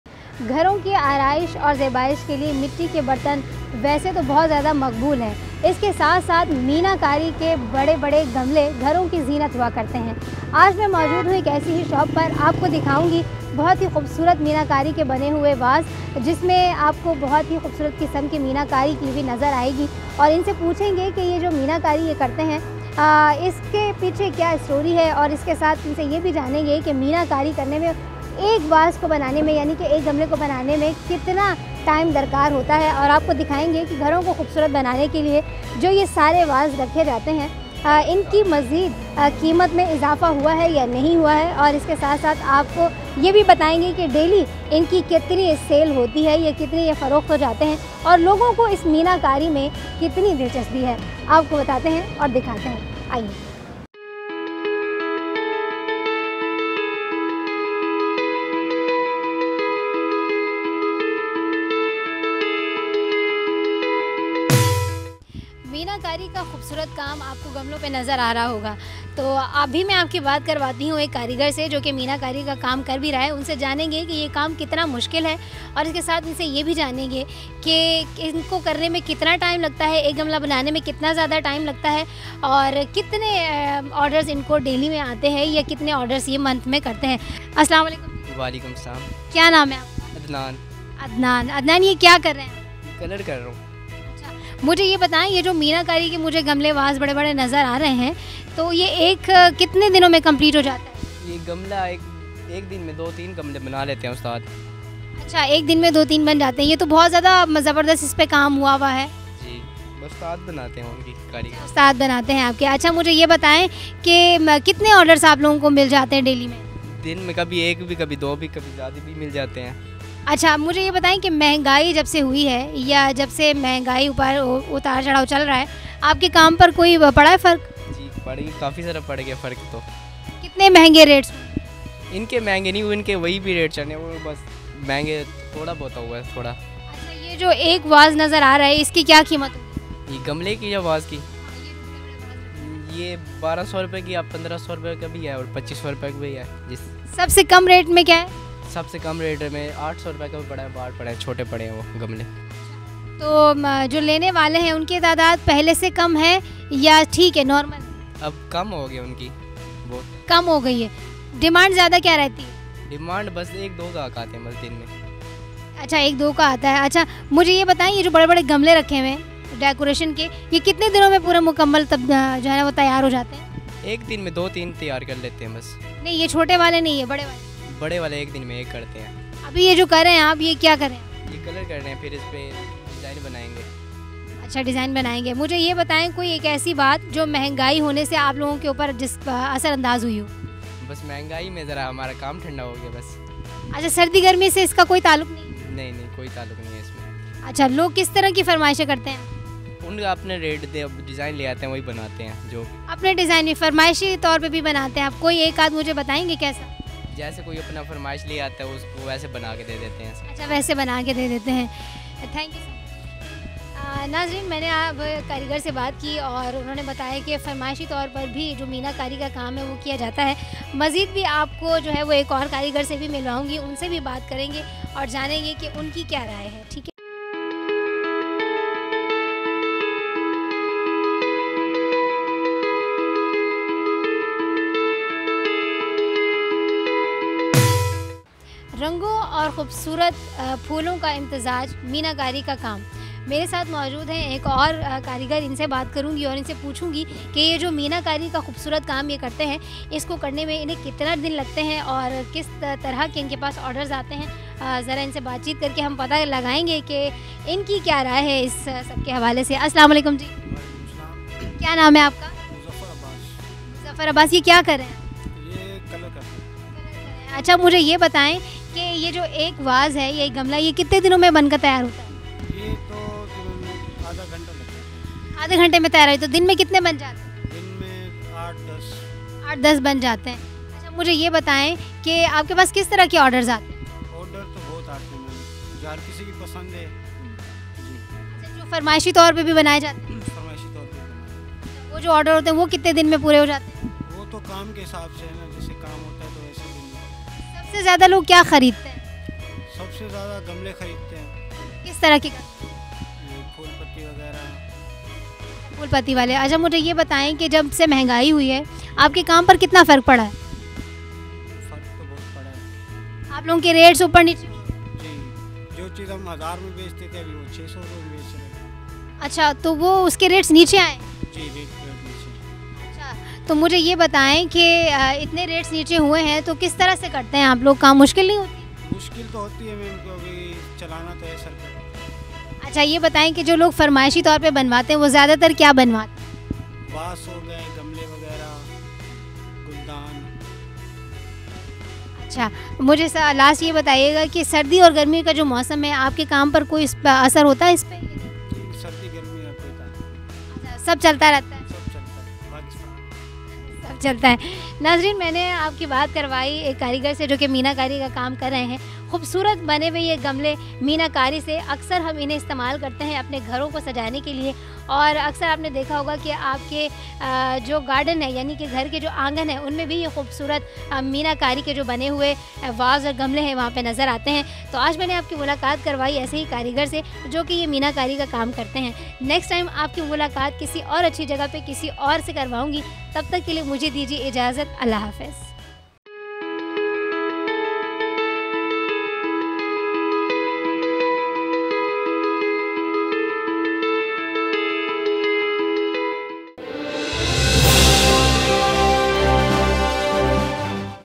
Enamel Pots
Learn about making of enamel pots with Komal
مینا کاری سے بنے ھوے گملوں کی تیاری کے بارے میں جانیے کومل کے ساتھ۔